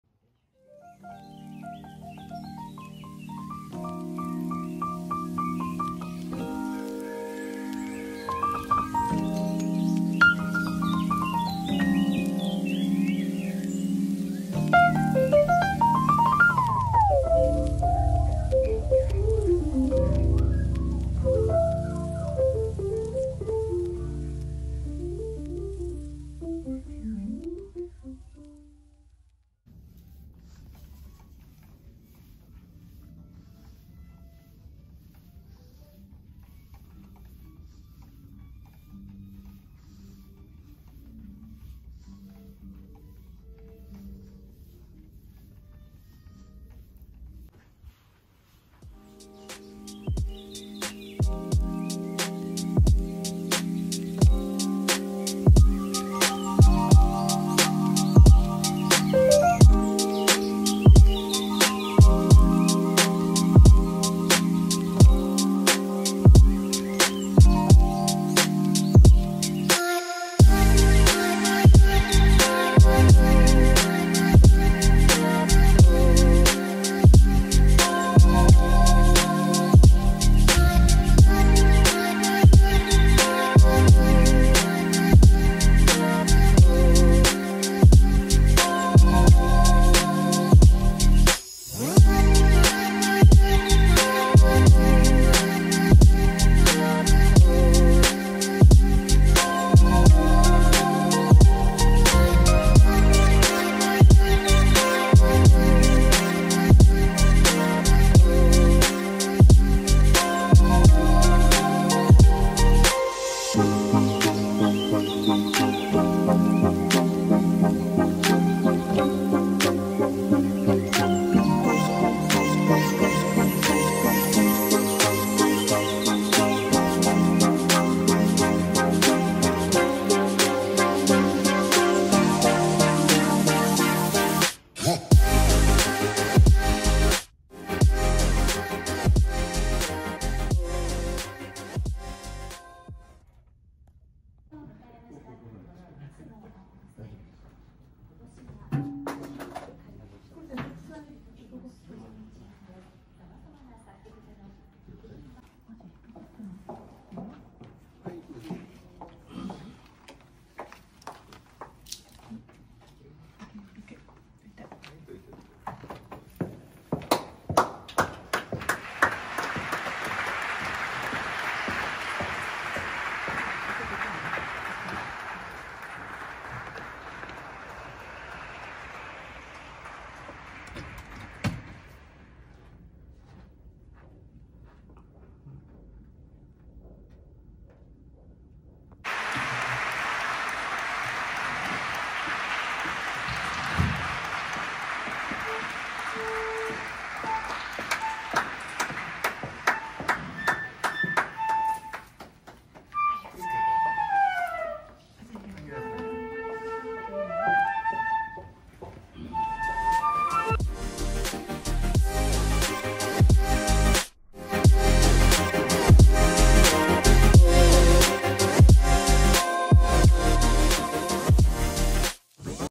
Summer Music Festival 2022 最終日 住友生命いずみホール 舞台裏編
Summer Music Festival 2022
全日開演17時
第3夜　住友生命いずみホール

今年で25回目を迎えるサマーミュージックフェスティバル大阪では、作品25特集を中心に記念年にあたる作曲家のフランク、フランセ、シマノフスキ、メシアン、クライスラー、ピアソラ、スクリャービン等の作品を織り交ぜながら3夜開催致します。